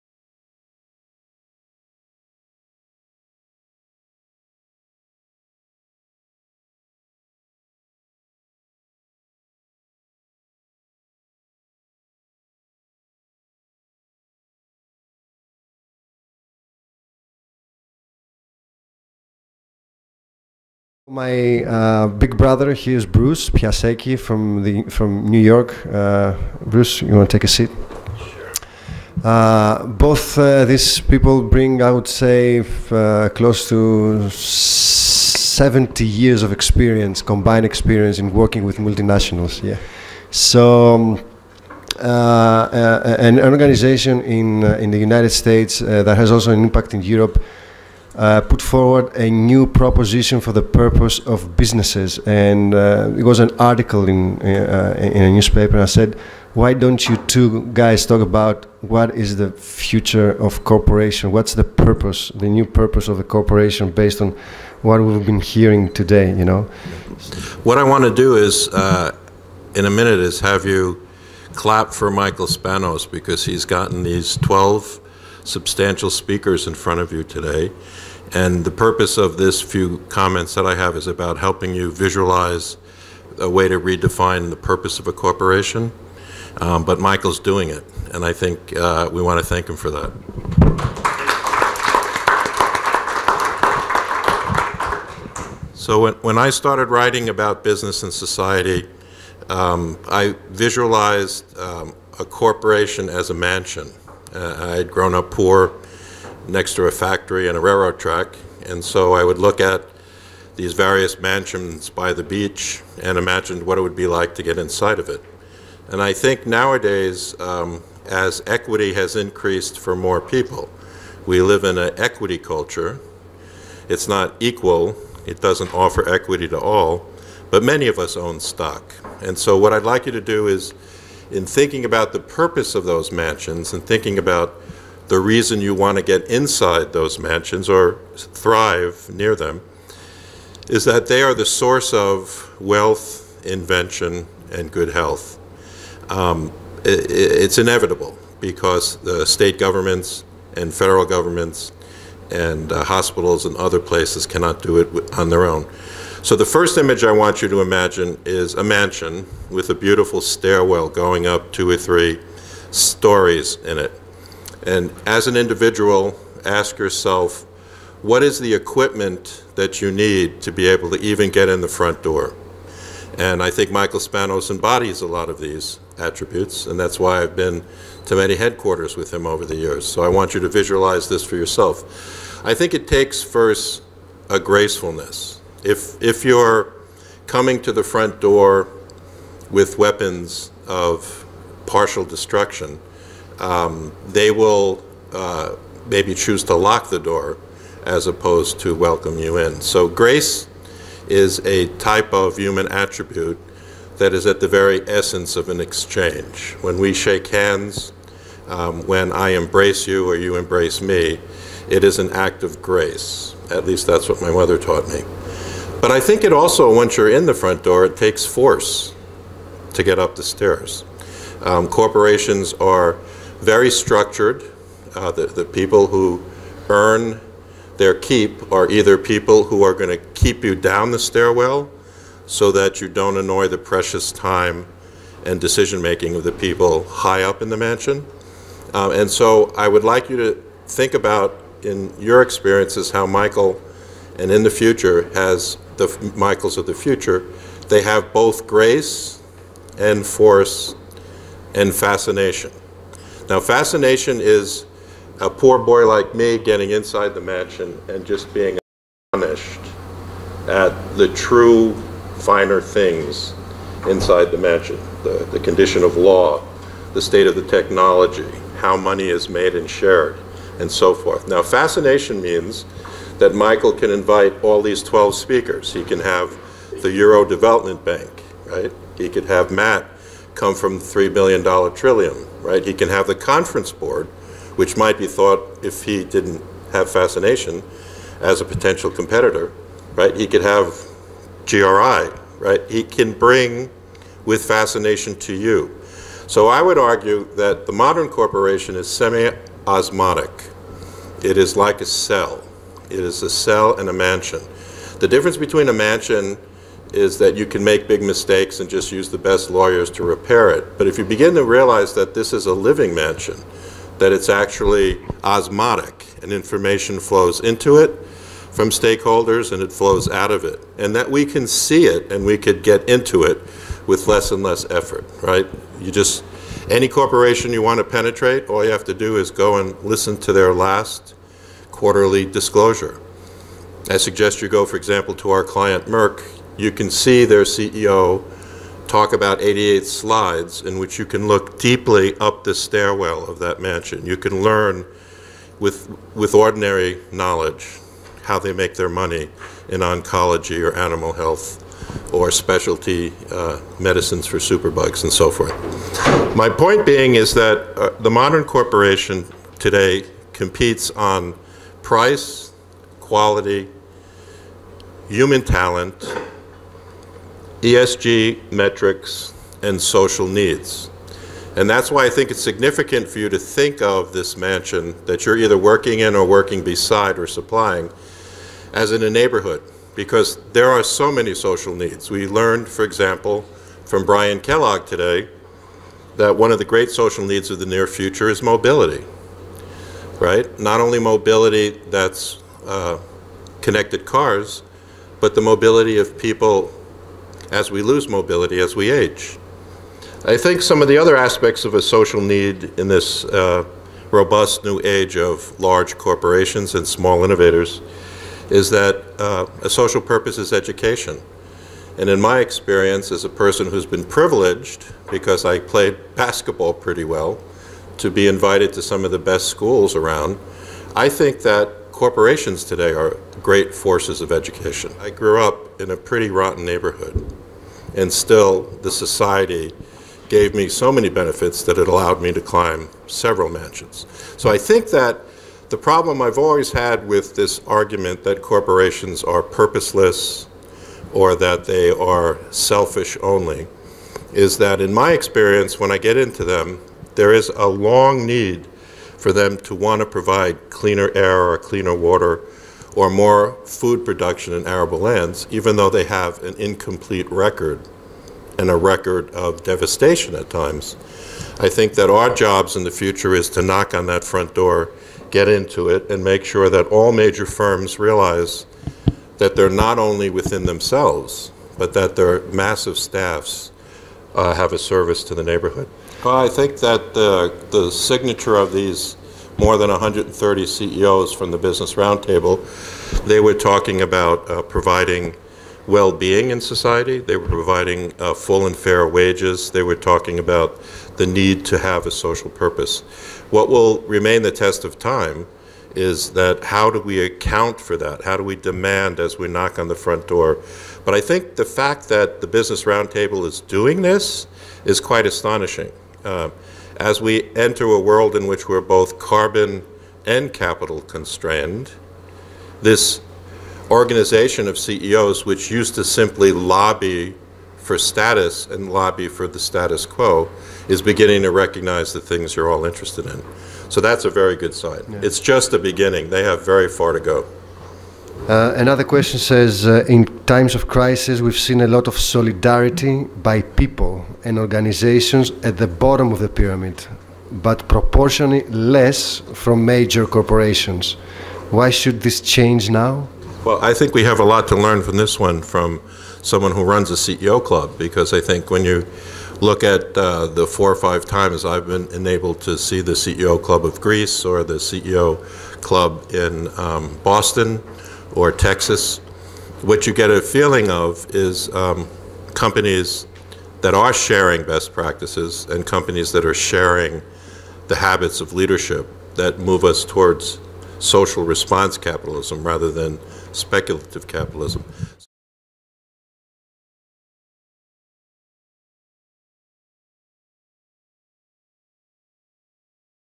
Redefining the Purpose of a Corporation
Excerpts from a Sustainability Forum panel discussion with Dr. Bruce Piasecki (Founder & President, AHC Group, Inc.) and Gerogia Kartsanis (Founder & CEO SARGIA Partners, Founder & President CEO Clubs Greece and Your Directors Club), held in Athens, Greece, on 4 October 2019 and hosted by Michael Spanos of Global Sustain.